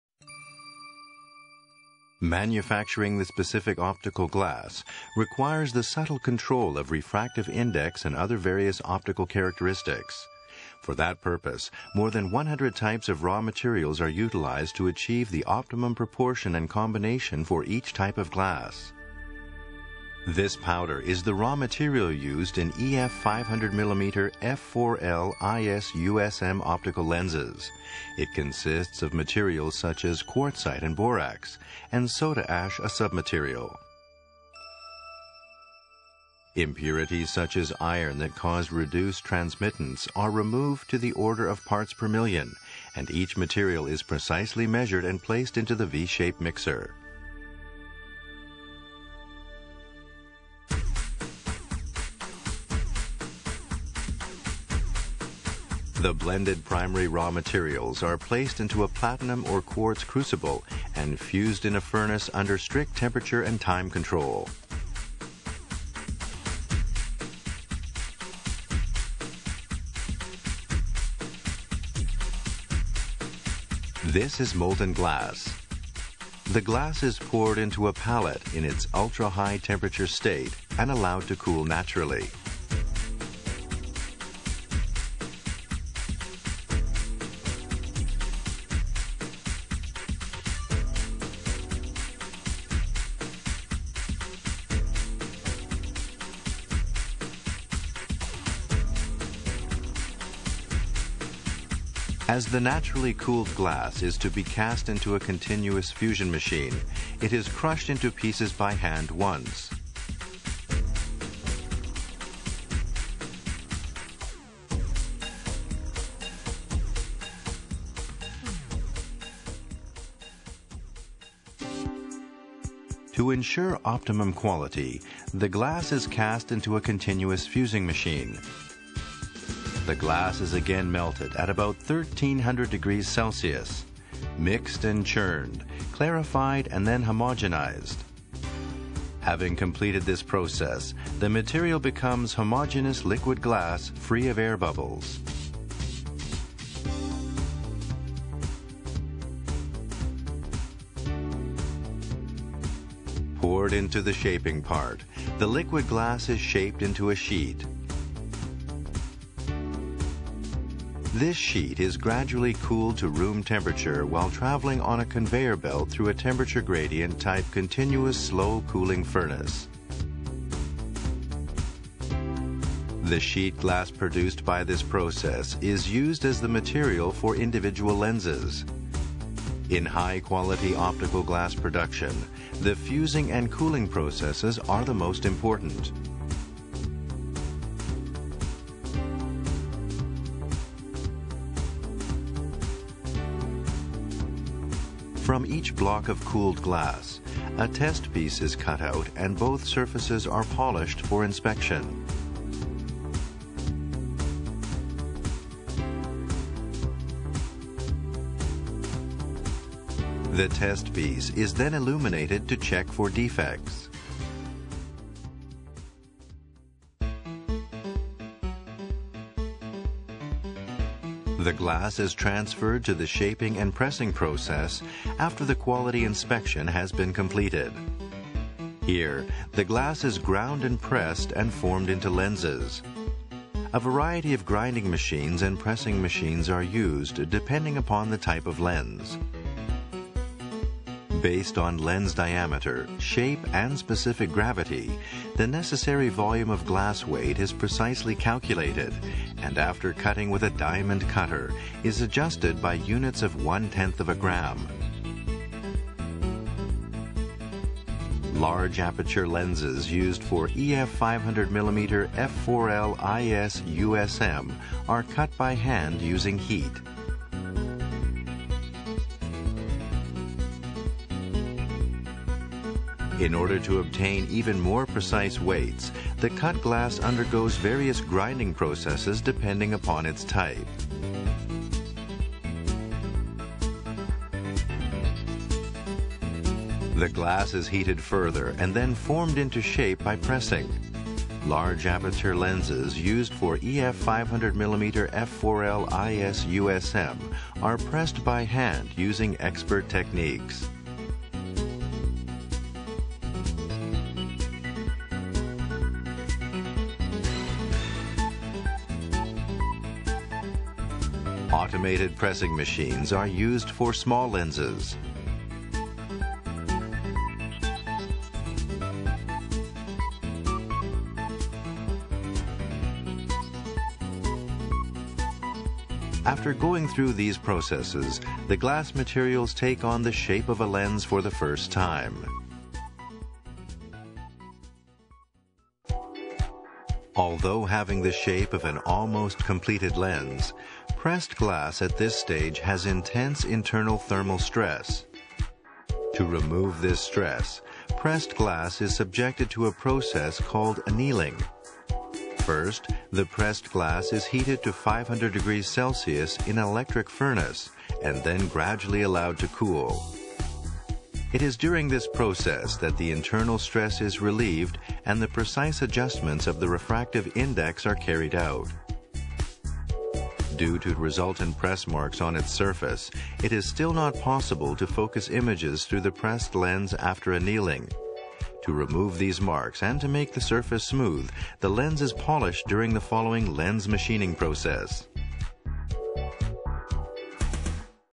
Canon EF Lens Plant - Material Processing Process
See how powdered raw materials are turned into materials for lenses. Take the opportunity to look at processes you are normally unable to see, such as a mixer blending raw materials and the spectacle of glass being poured. Parts: 1. Material blending, 2. Pre-Fusing 3. Fusing/Cooling, 4. Quality inspection, 5. Press / Forming, 6. Annealing